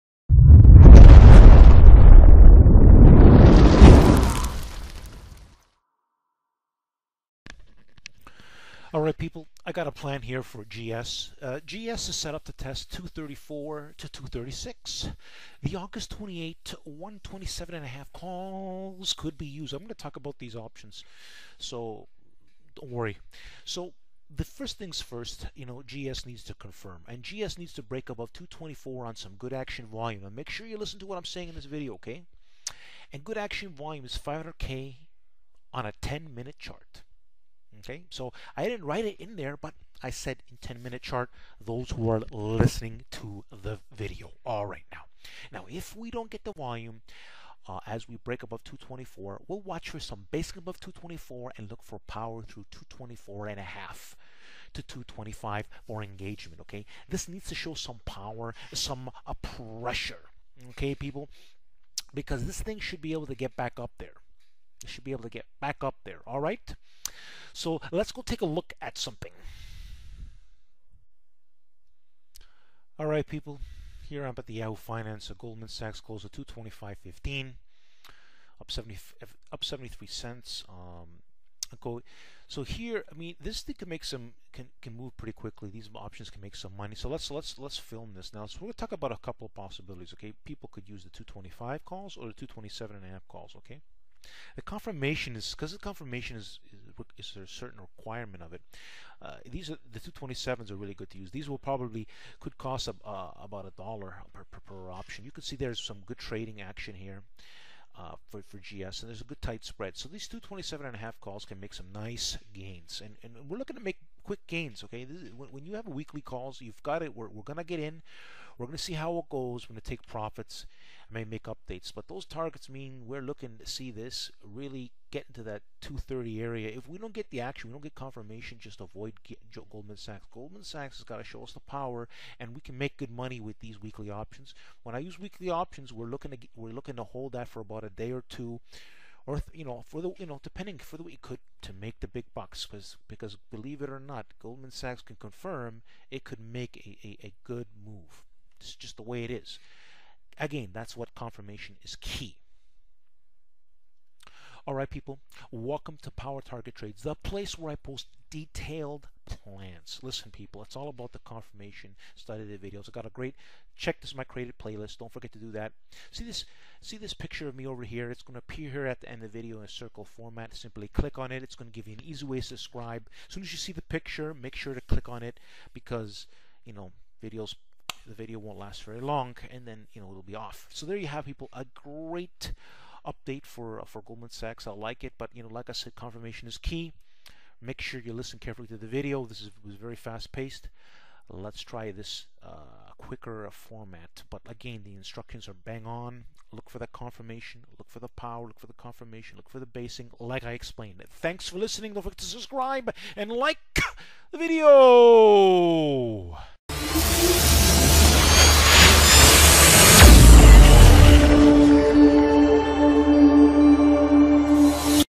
HOW TO TRADE OR BUY OPTIONS FOR GREAT GAINS
I explain an options plan for $GS. confirmation is key video explain a lot of details before we engage in the options make sure to listen to all the details this is a quick video but with powerful advice